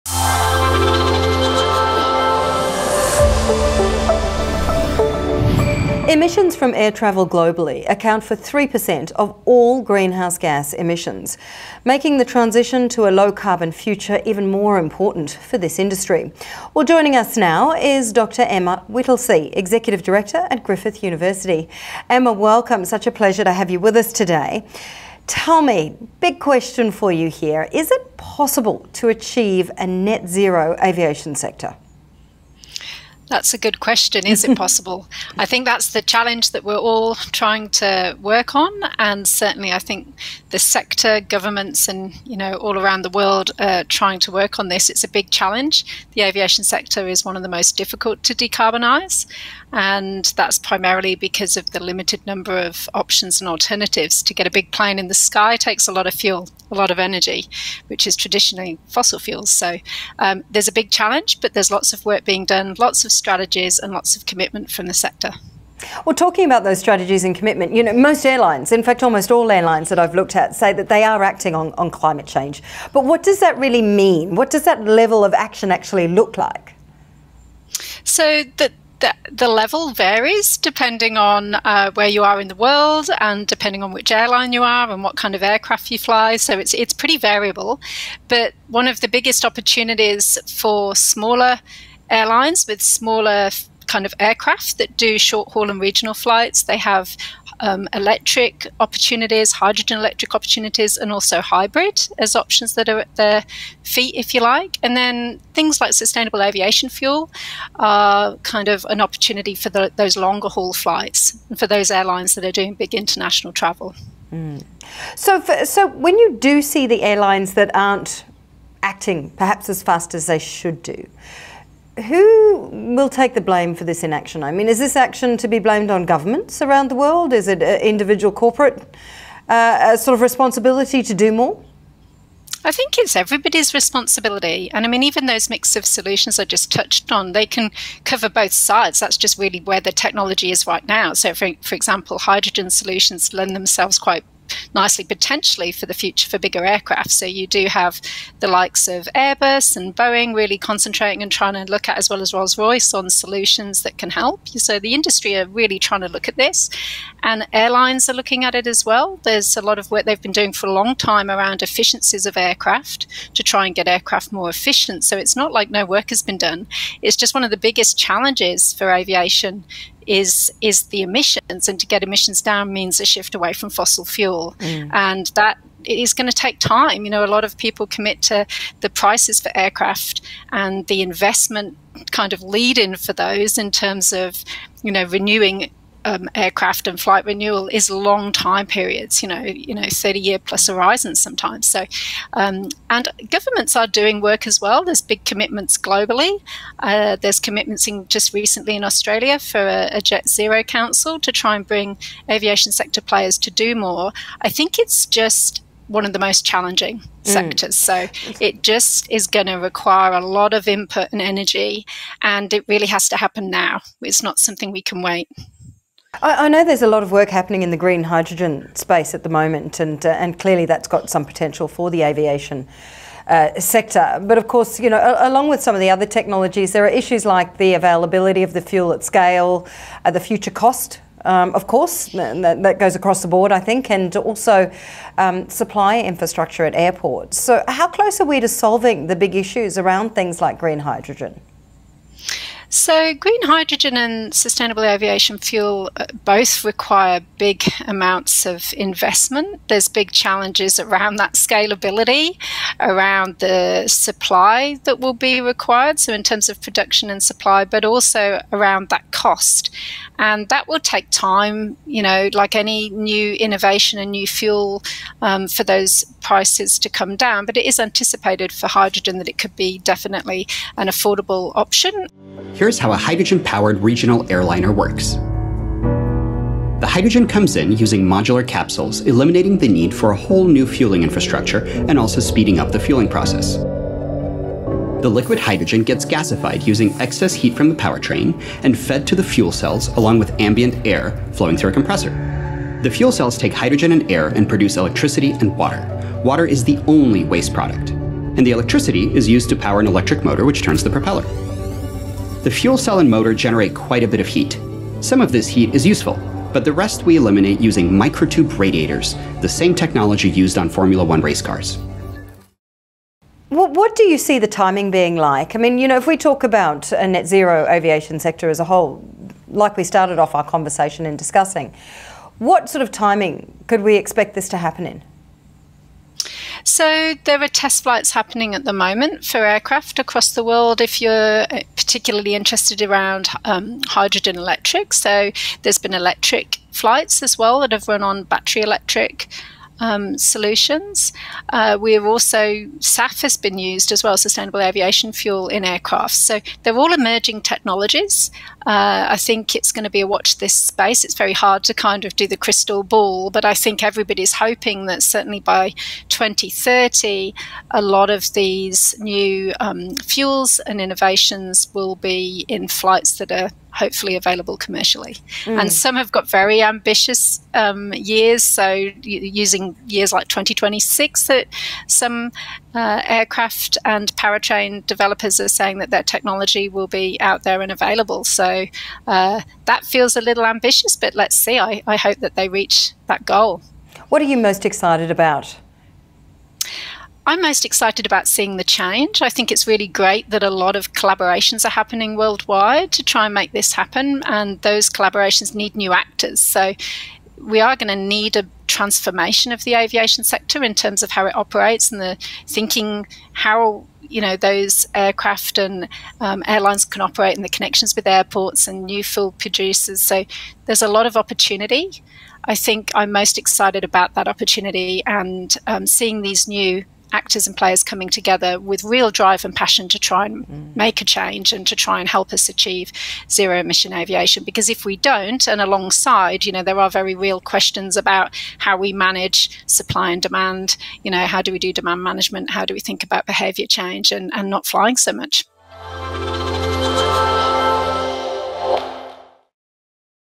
Can aviation ever reach net zero emissions?
The aviation industry is one of the most challenging sectors to decarbonise.
On the road to net zero, it will be one of the great triumphs if carbon neutral flights can be achieved by 2050.
Dr. EMMA WHITTLESEA from Griffith University discusses the reality and the options ahead for renewable energies powering aircraft.